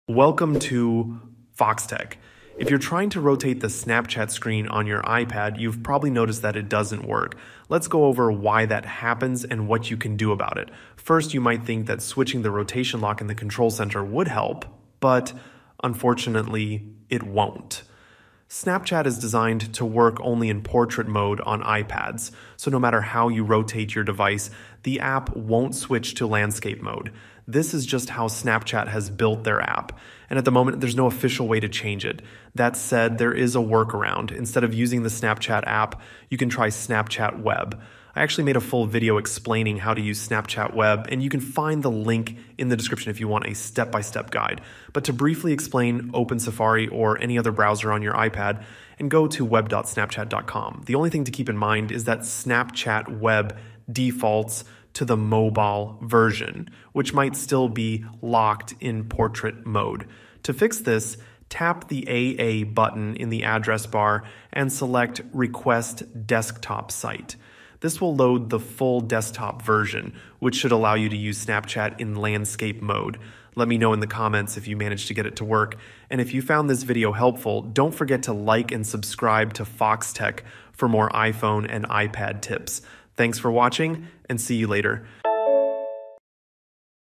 How to Rotate Snapchat Screen on iPad (explained)
As you may or may not know the iPad's version of Snapchat only works in the portrait orientation and it's kinda hard to rotate it sideways, but there is one way how you can achieve it.